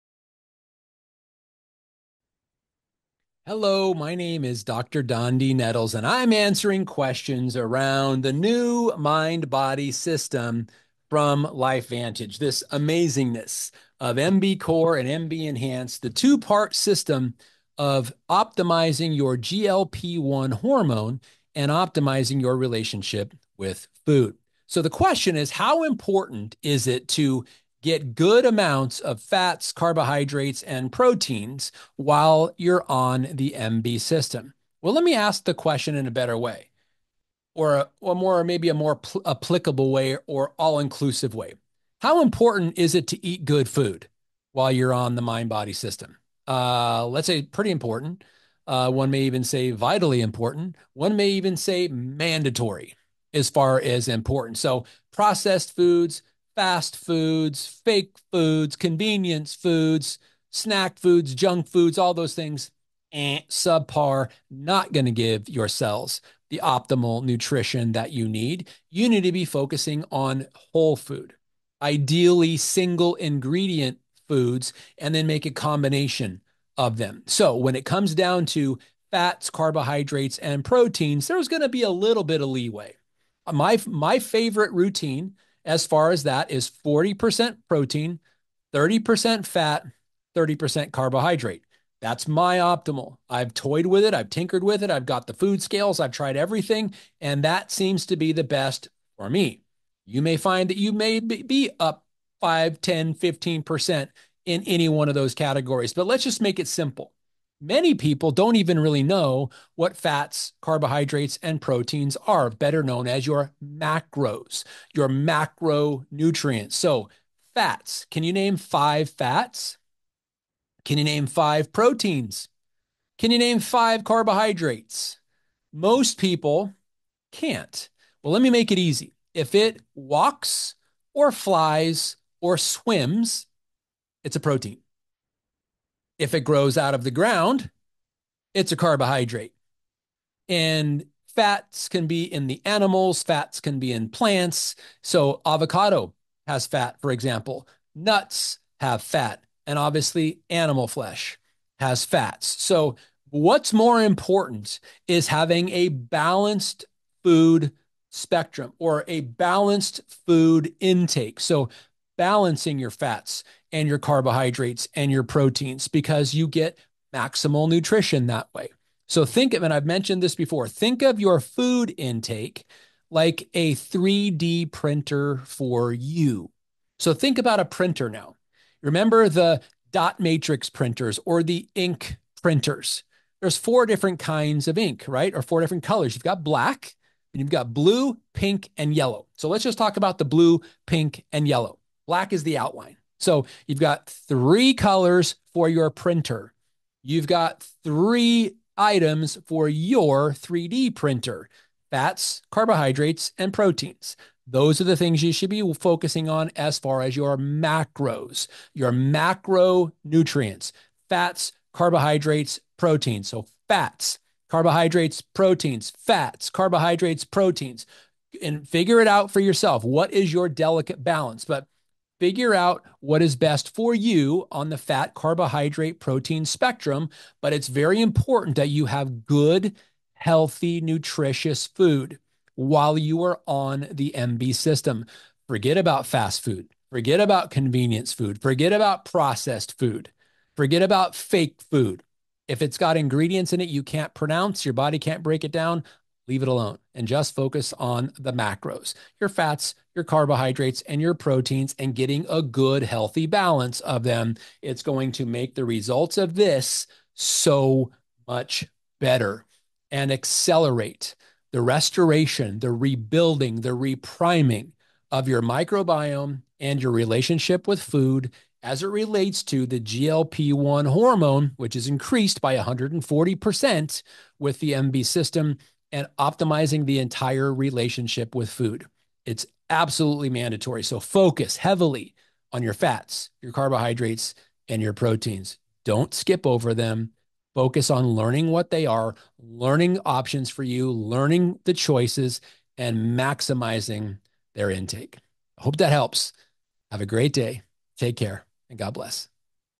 How Important is Getting Enough Fat, Carbs & Protein on a Daily Basis, with the MB System?
How Important is Getting Enough Fat, Carbs & Protein on a Daily Basis, with the MB System? with Dr. Dondee Nettles